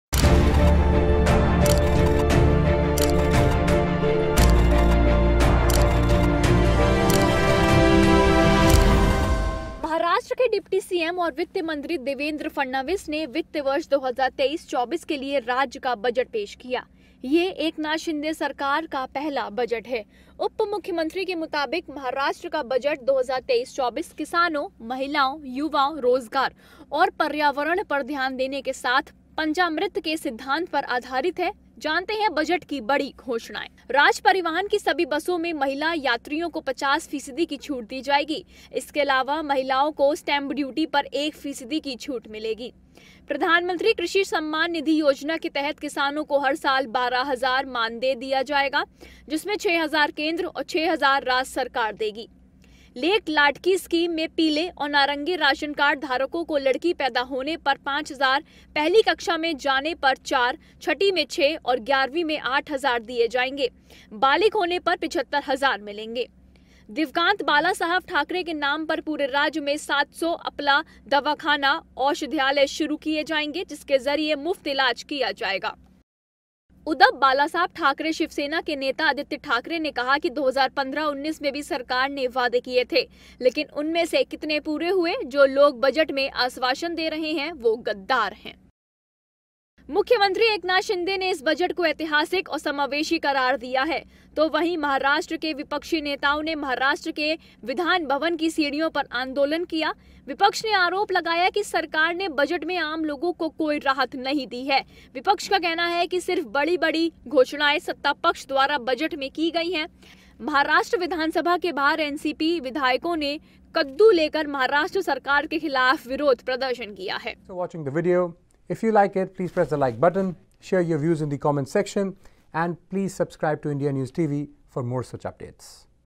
Oppn Mlas Stage Protest Over Maharashtra Budget Outside Vidhan Bhavan
The Opposition party MLAs Friday staged a protest outside Vidhan Bhavan over Maharashtra Budget presented in the Assembly a day earlier. Terming the Budget as a “populous election Budget,” the legislators raised slogans by holding pumpkins in their hands. Earlier on Friday, Shiv Sena (UBT) leader Aaditya Thackeray criticised the Maharashtra government over the state Budget. He said even as assurances have been given to all, the question arises how many of them will be fulfilled